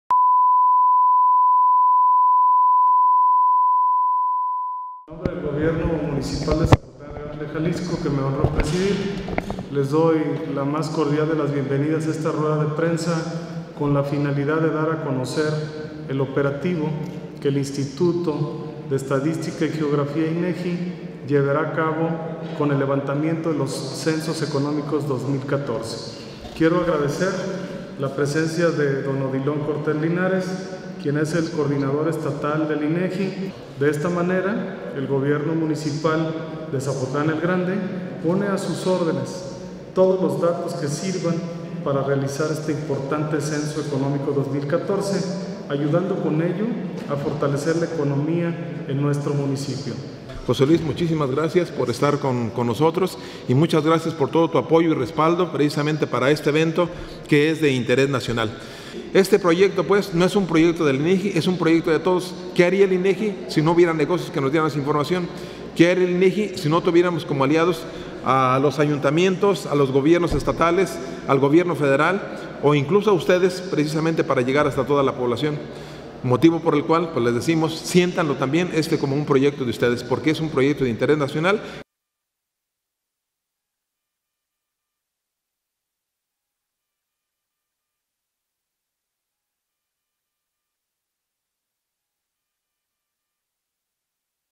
Rueda de prensa | Censos Económicos 2014 en Zapotlán el Grande
Vídeo informativo del departamento de Comunicación Social del Gobierno Municipal de Zapotlán el Grande, Admón. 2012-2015, disponible en HD.
Noticia del día: Agradece representante de INEGI apoyo del presidente municipal José Luis Orozco Sánchez Aldana, durante rueda de prensa convocada para dar a conocer el proyecto nacional de los "Censos Económicos 2014".
Fue el pasado 4 de febrero cuando arrancó en el municipio la edición número 18 de los Censos Económicos (CE) de México, que permitirán conocer la información estadística básica de todos los establecimientos productores de bienes, comercializadores de mercancías y prestadores de servicios para generar indicadores económicos del país a un gran nivel de detalle geográfico, sectorial y temático.
En el estado de Jalisco se contará con la participación de un equipo de 1,547 personas, entre los meses de febrero y julio de 2014, los cuales estarán ubicados en 160 oficinas, las cuales han sido ubicadas principalmente en las ciudades medias del Estado.
En diciembre de este año se publicarán los resultados oportunos de los CE 2014, mientras que los definitivos generales estarán disponibles en julio de 2015.